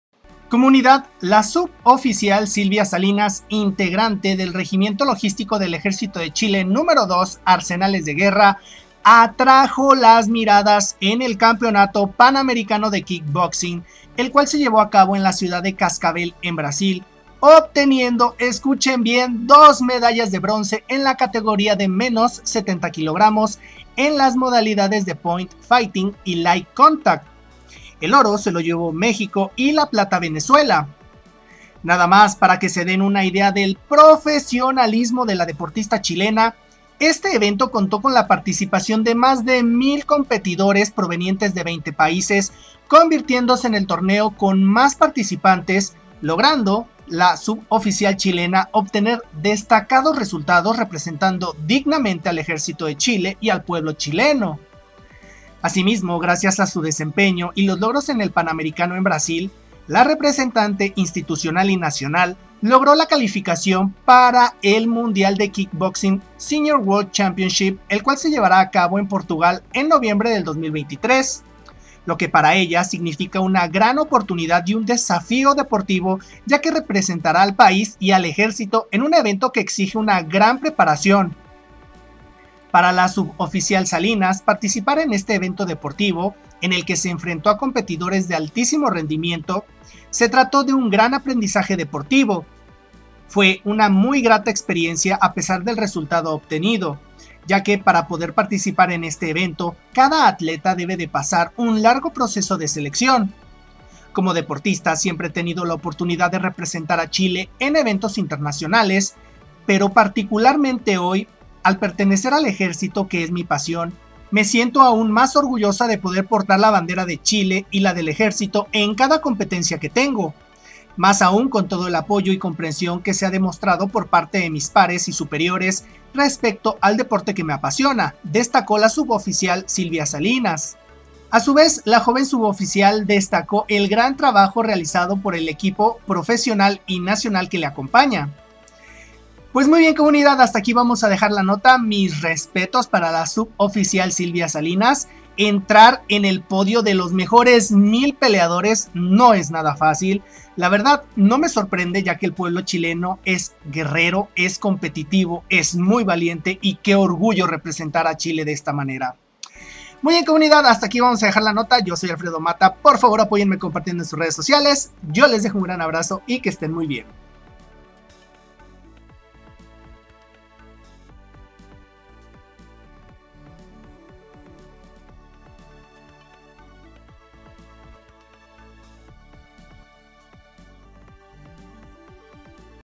Sub-Oficial del Ejército de Chile GANA Campeonato de Kick Boxing 🇨🇱
*Videos Imperdibles... 🇨🇱 CACHAI?*

~ Alfredomatta